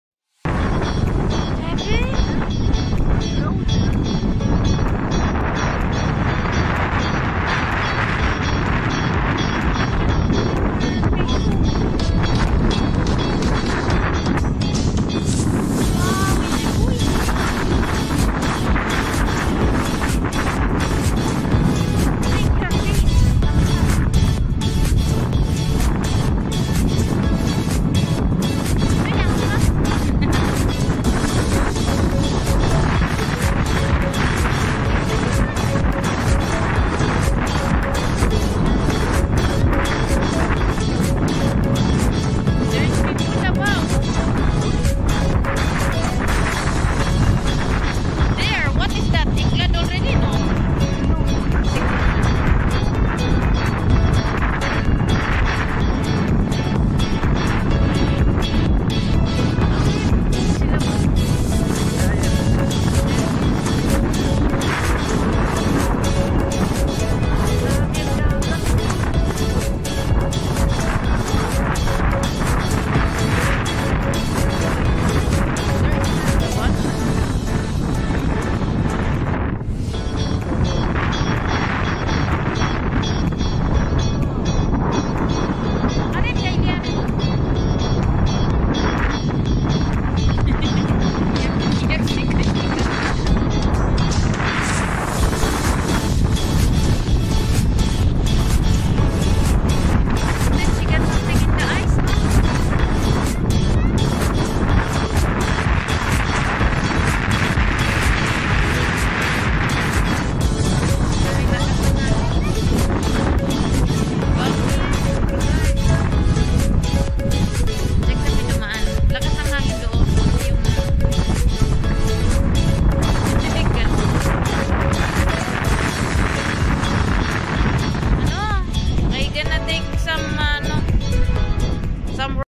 Potbail Sur Mer France
Lakas NG hangin sa gilid NG dagat